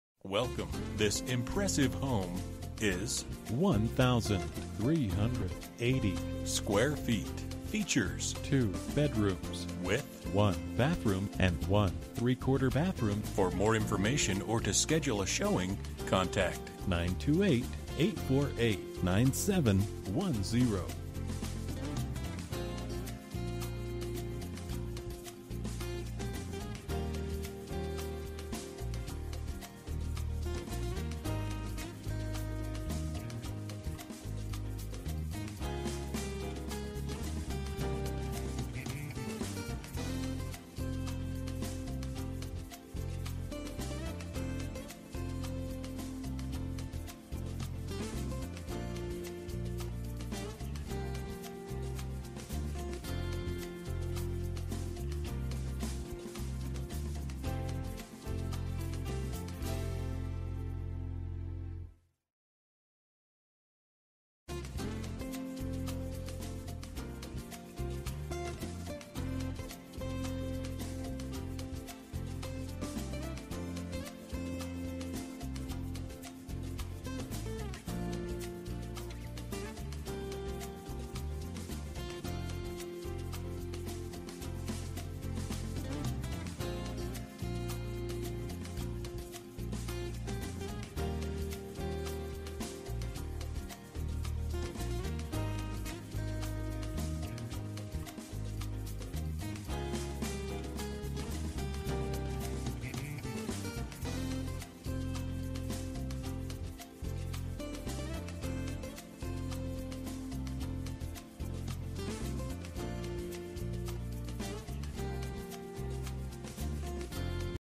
Residential for sale - 3101 Simpson Lane, Prescott, AZ 86301
Don't miss this great home in the highly desired Lakeview Estates. Enjoy views of the lake and Granite Mountain on your large covered patio. Home purchased in 2015 and seller added a new roof, new skylights, new chain link fence around back yard, new paint, new flooring, new carpet in the bedrooms, and a new AC unit. Wow! The Arizona Room and carport are permitted and the home has been certified and engineered (see documents tab). Such a great value for this property it won't last long.

Bedrooms: 2
Bathrooms: 1 full and 1 three-quarter
Square Feet: 1380
Price: $181,900